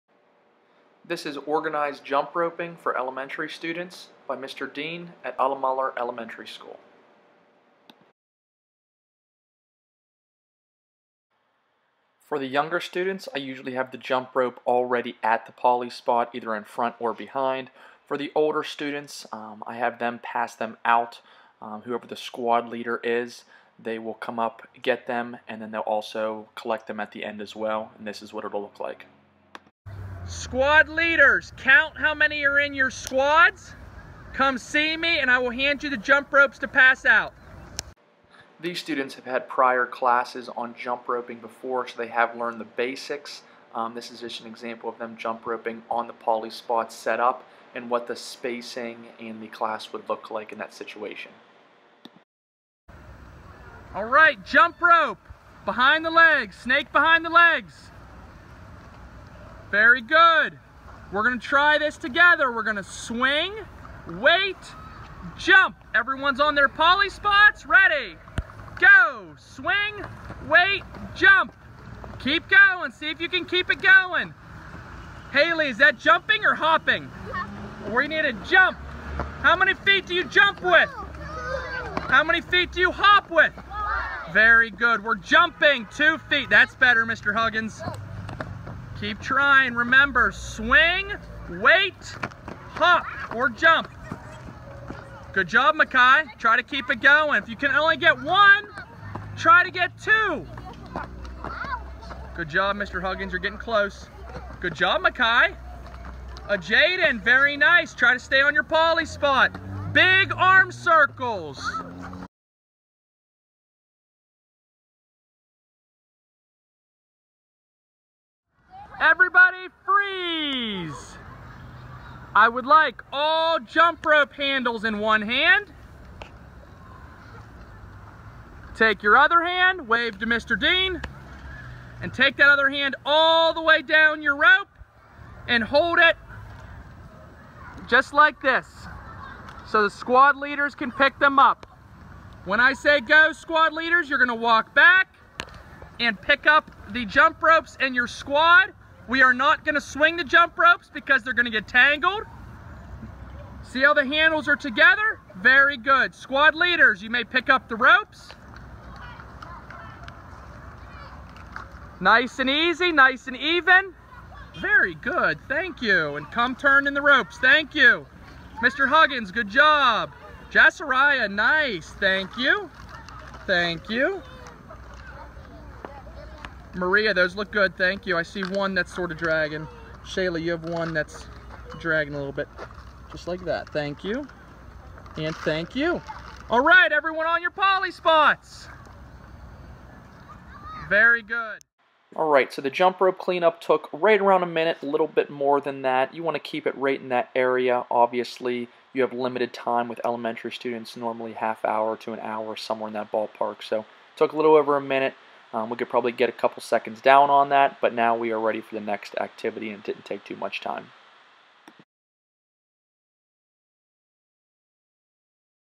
Elementary Physical Education - Jump Roping
This shows some basics of instructing an elementary PE class.